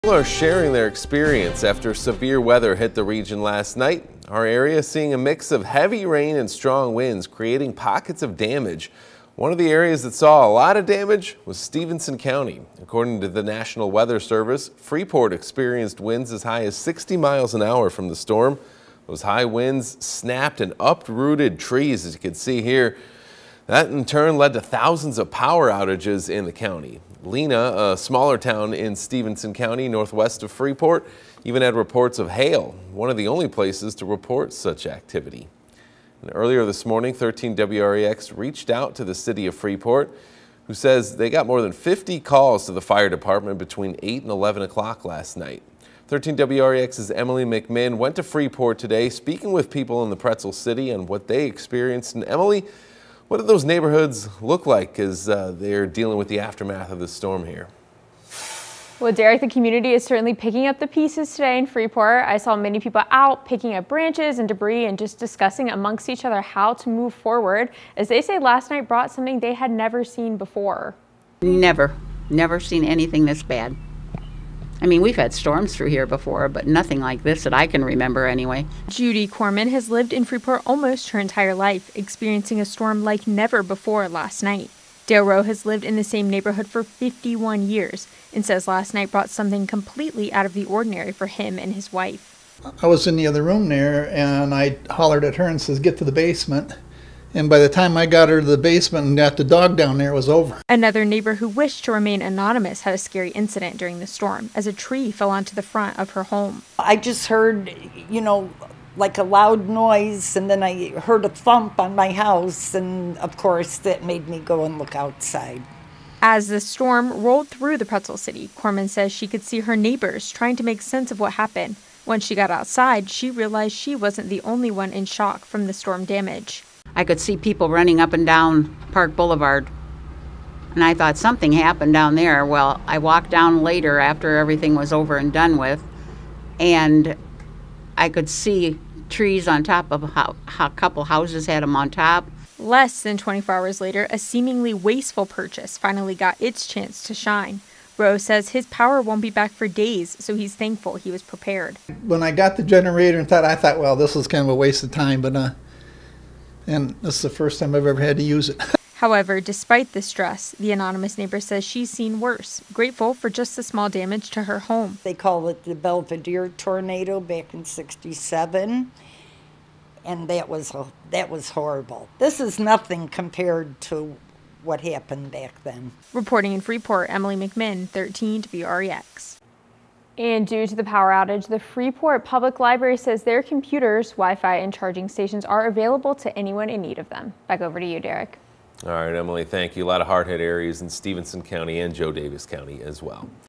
Freeport residents share their reactions to Tuesday's storms as they pick up the pieces
City staff is now stepping in doing what they can to help residents.